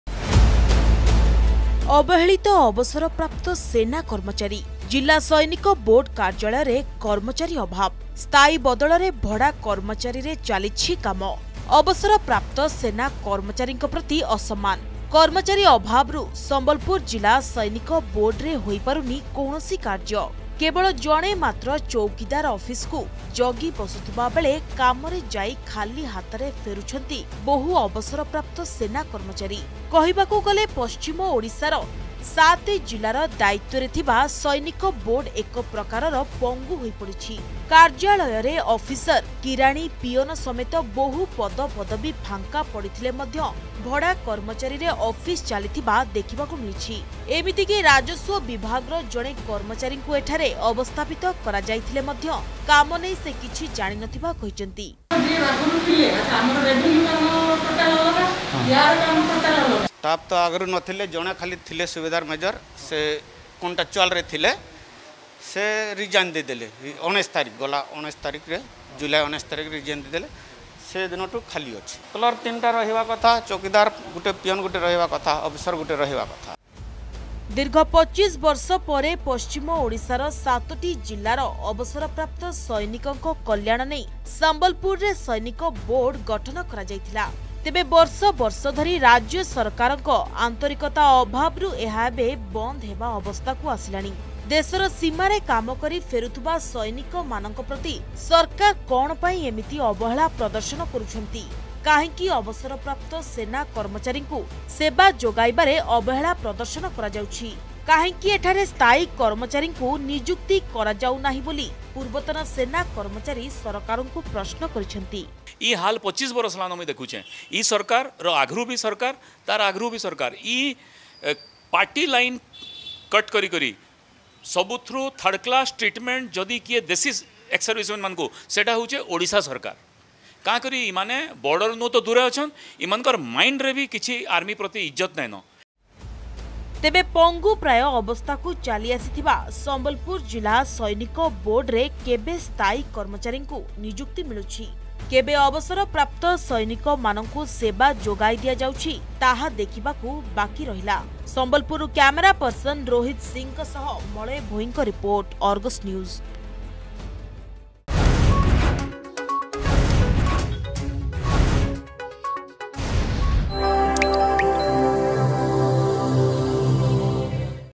ଅବହେଳିତ ଅବସରପ୍ରାପ୍ତ ସେନା କର୍ମଚାରୀ
ଅବହେଳିତ ଅବସରପ୍ରାପ୍ତ ସେନା କର୍ମଚାରୀ। ଜିଲ୍ଲା ସୈନିକ ବୋର୍ଡ କାର୍ଯ୍ୟଳୟରେ କର୍ମଚାରୀ ଅଭାବ। ସ୍ଥାୟୀ ବଦଳରେ ଭଡା କର୍ମଚାରୀରେ ଚାଲିଛି କାମ।